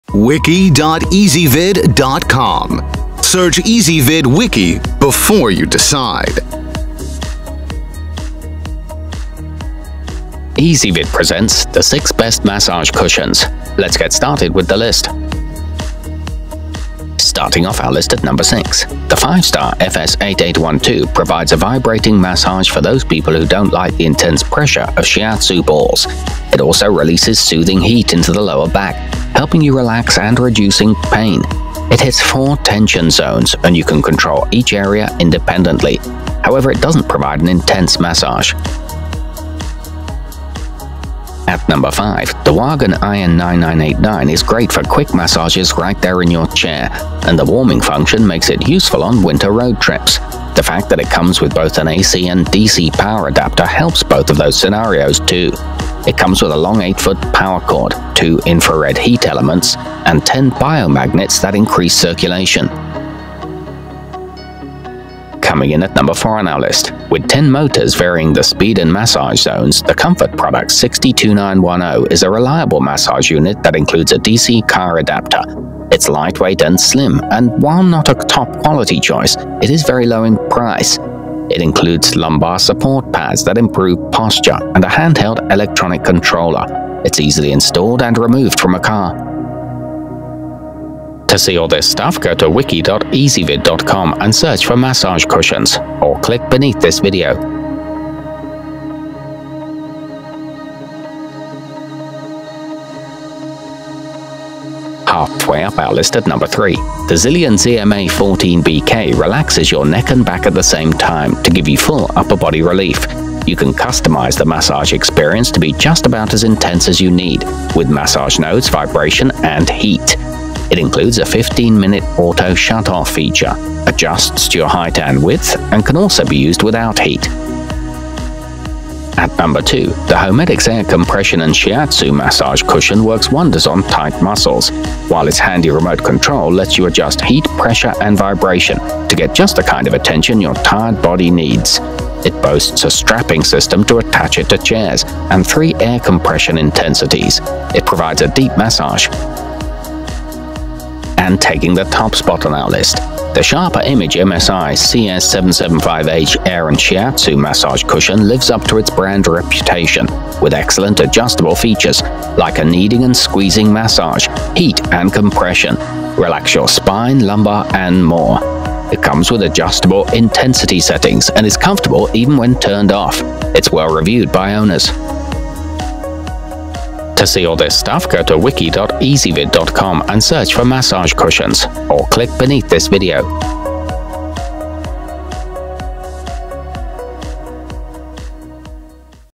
6 Best Massage Cushions 2016
Massage Cushions Reviewed In This Wiki:
FIVE STAR FS8812
Wagan IN9989
Comfort Products 60-2910
Zyllion ZMA-14-BK
Homedics Air Compression and Shiatsu
MSI-CS775H Air and Shiatsu